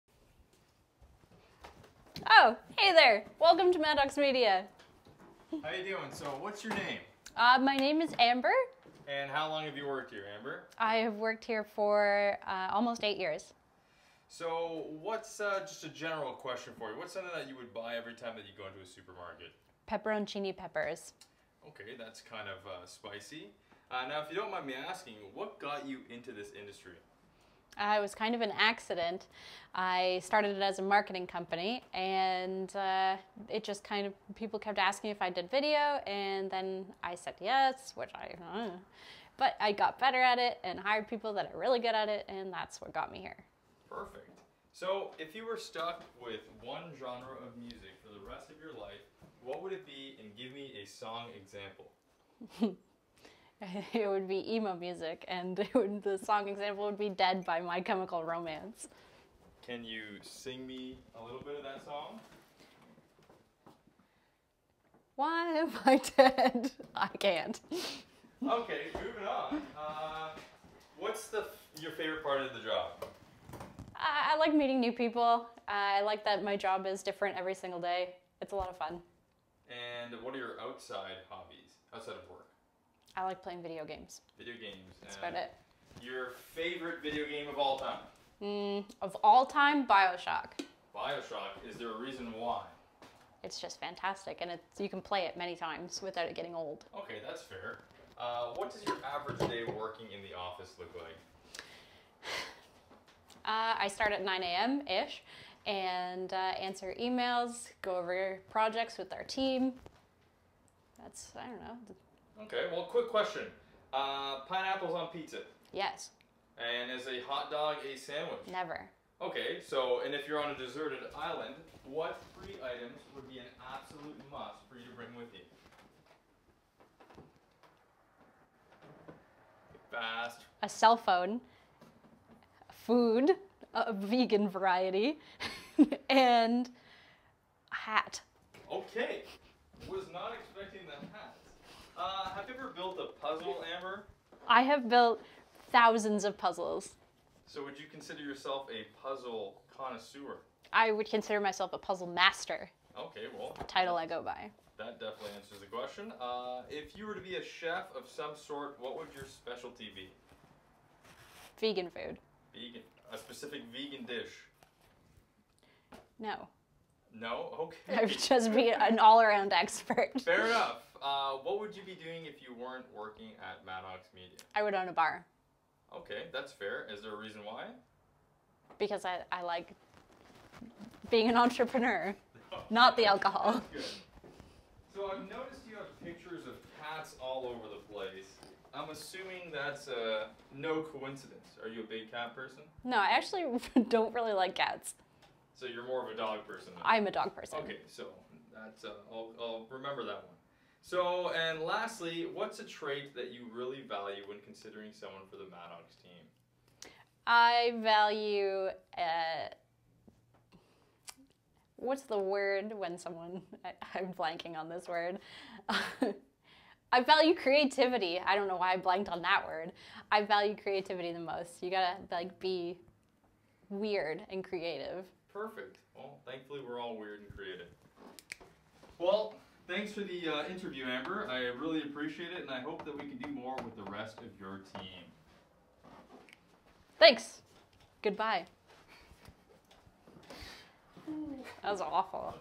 Questions With Amber - Maddox Media
"I value creativity!"

Meet Amber, the founder and owner of Maddox Media. It was a blast asking her about her work and day to day life with a mix of some interesting curveball questions...Stay tuned for more questions with the rest of us here at Maddox Media!